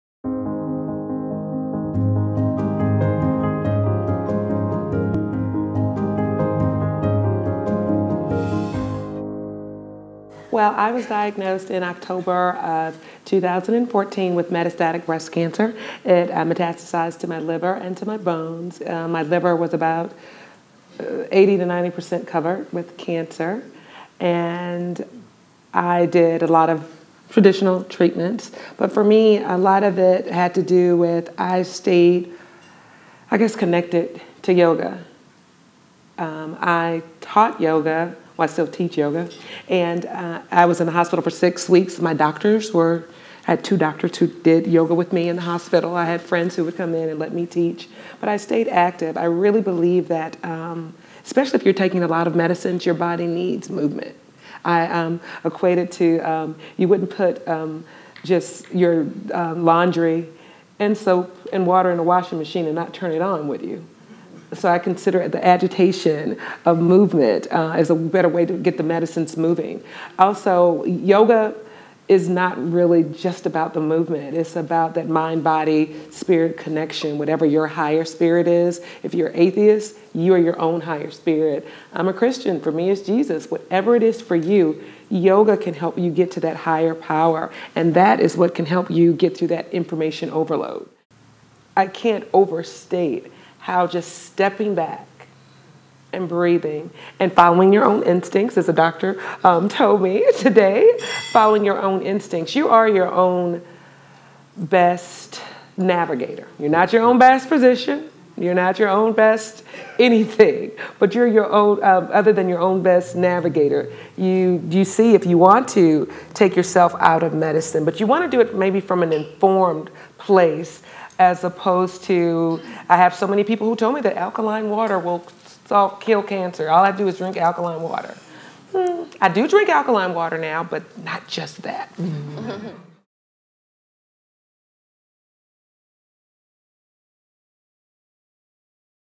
One Woman's Story of Finding What Works for Her
Lorenita Lucas discusses her process of finding what practices increase her sense of well-being.